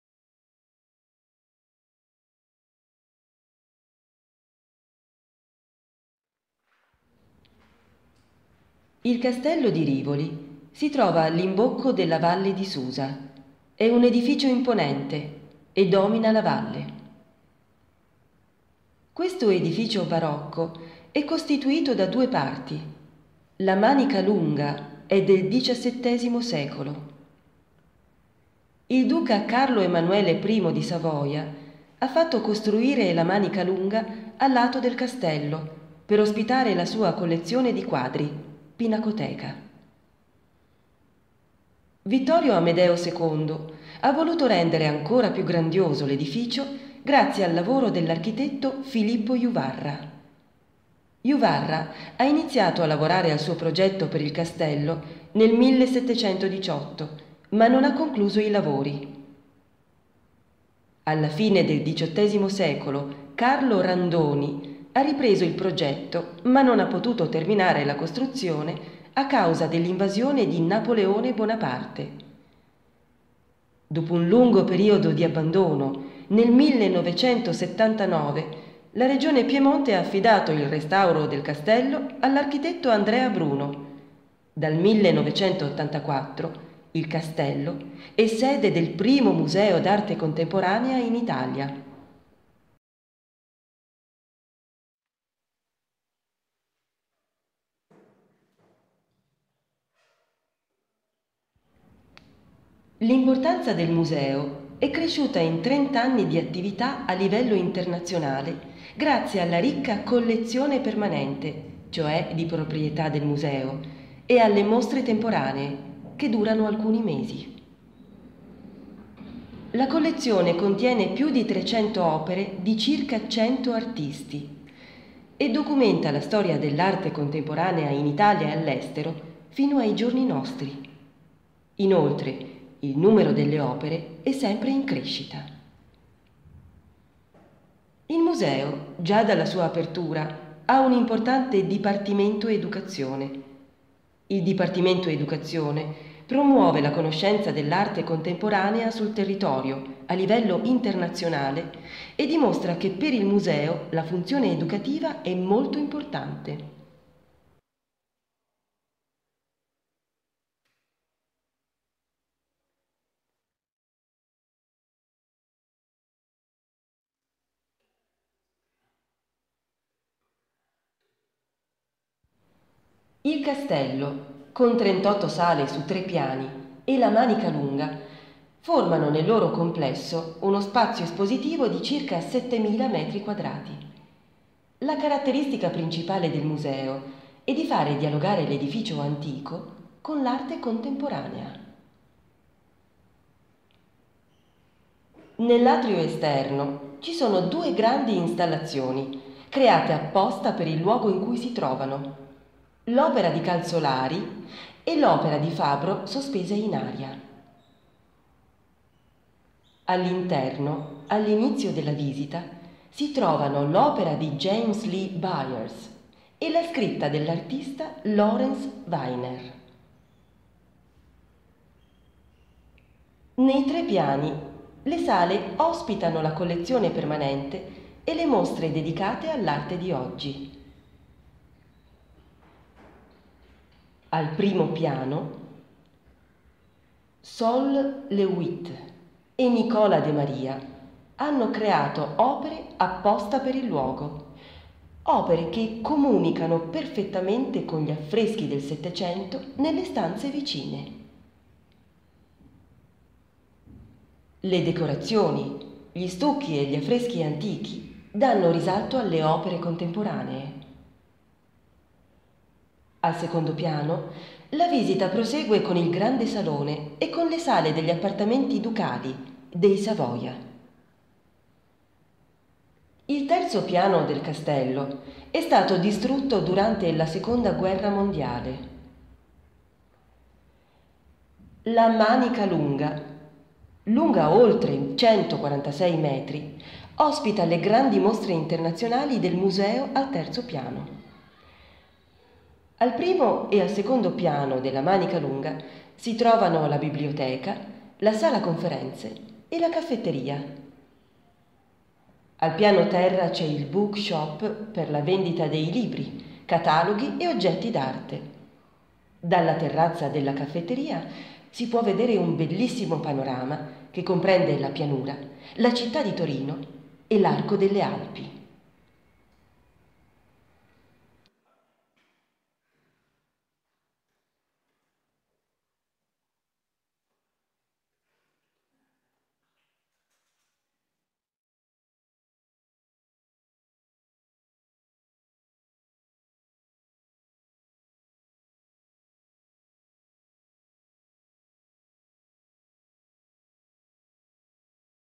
Il castello di Rivoli e il Museo d'Arte Contemporanea
Video in Lingua dei Segni Italiana (LIS) con sottotitolazione, voce e immagini.
Video per l'accessibilità culturale.